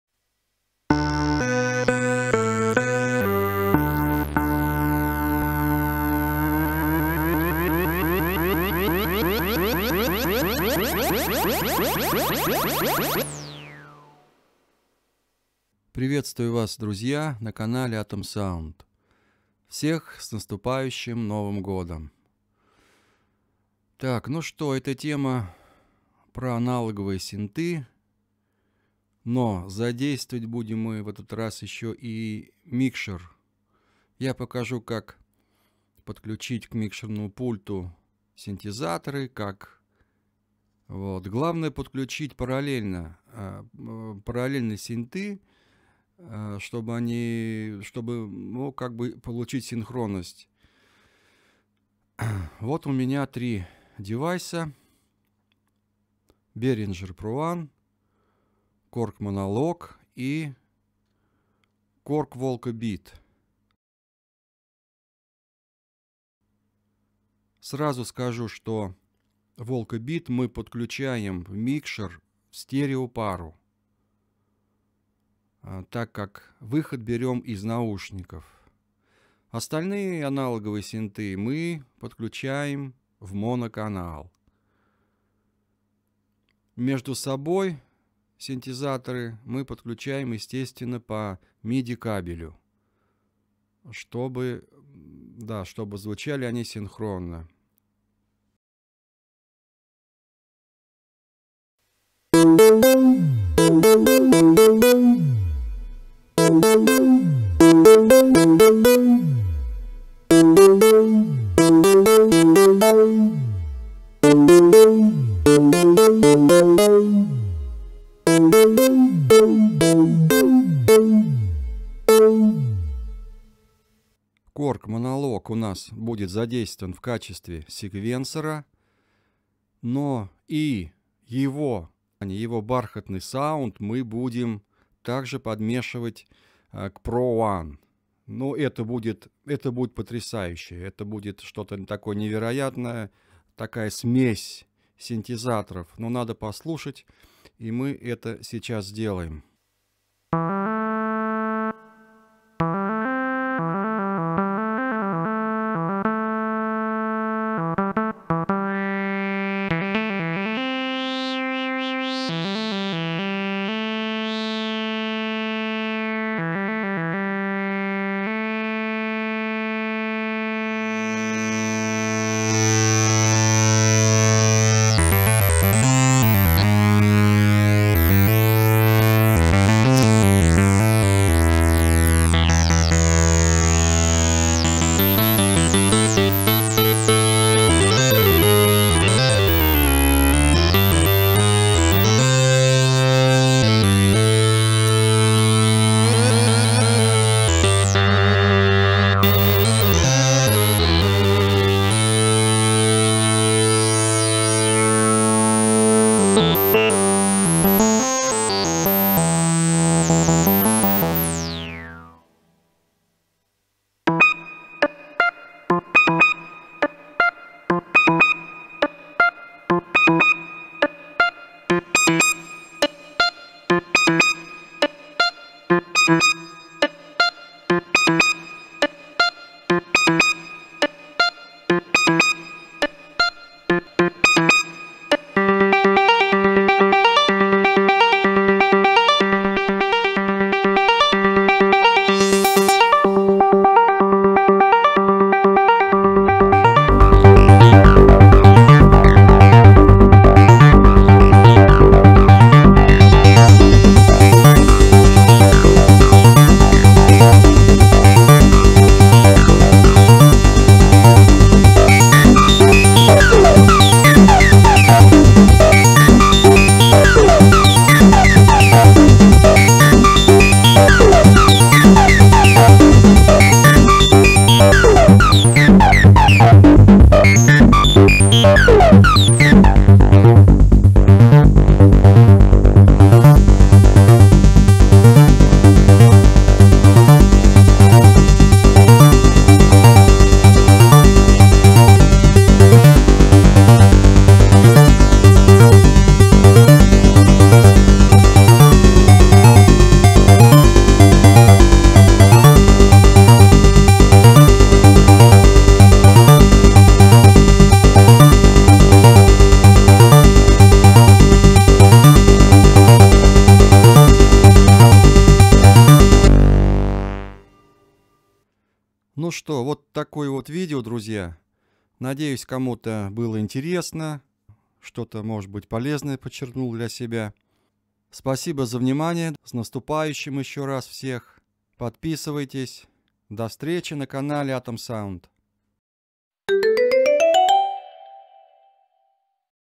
Микшируем синтезаторы_получаем интересный саунд
ИТОГ: Интересный саунд!
Микшируем синтезаторы - задействуем аналоговые синты и все это через микшерный пульт. Соединяем их с собой по миди-кабелю. К микшеру подключаем в моно-каналы. Подмешиваем звук по вкусу -  Получаем невероятный саунд! В видео участвуют Behringer Pro-1 & Korg monologue , но это могут быть любые синтезаторы, главное правильно подключить. Смотрите видео! Есть вопросы, спрашивайте!